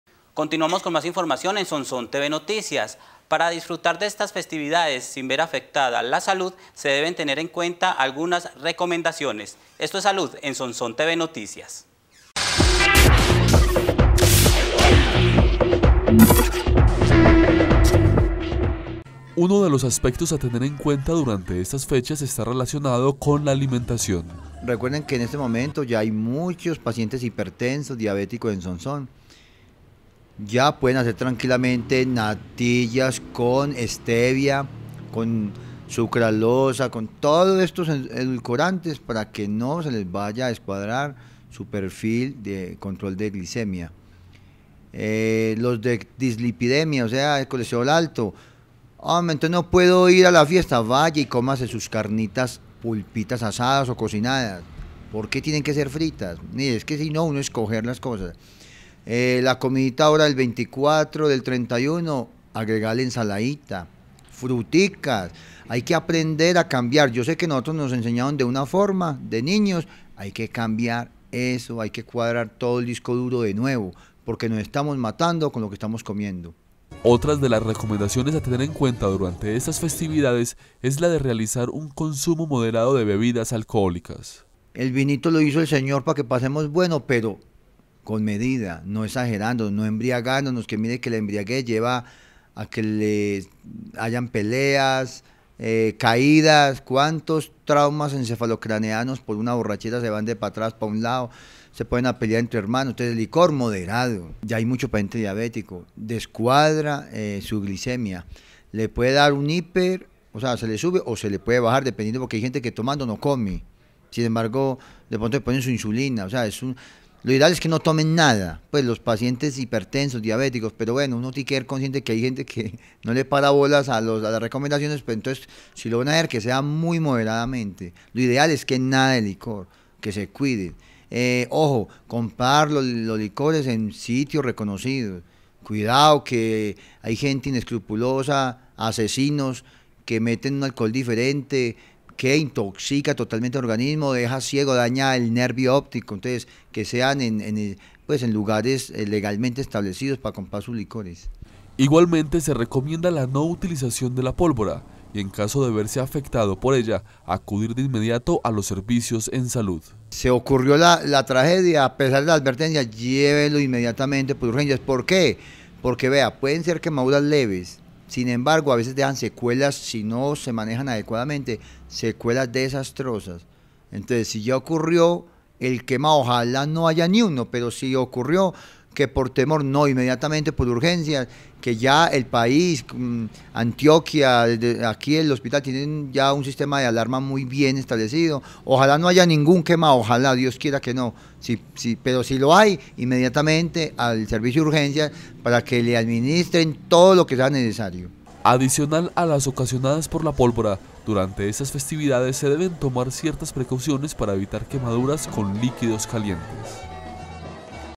Recomendaciones de salud para las fiestas de fin de año
Para poder disfrutar de estas festividades sin ver afectada la salud se deben tener en cuenta algunas recomendaciones